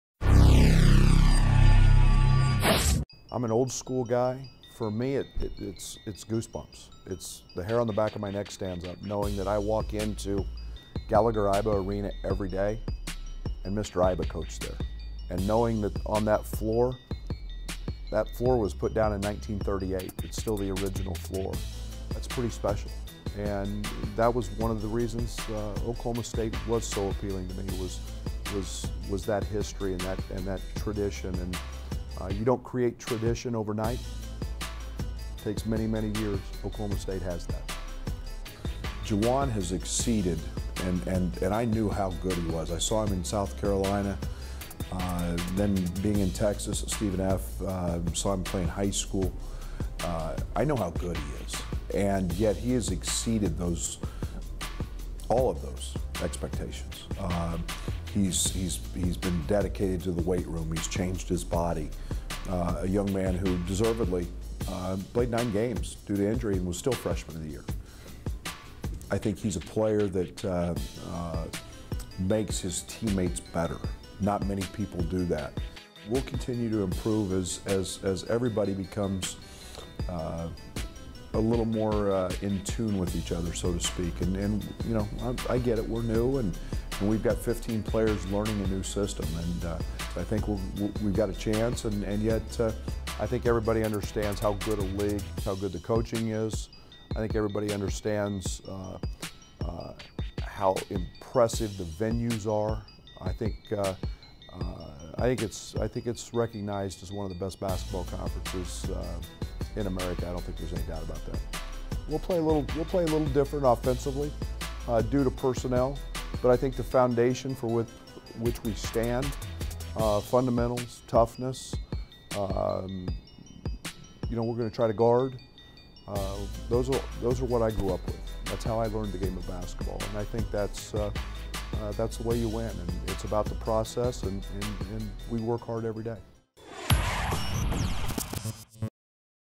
2016-2017 Oklahoma State Men's Basketball Preview
First year head coach Brad Underwood discusses why he's most excited about the upcoming season and the key players to watch this year.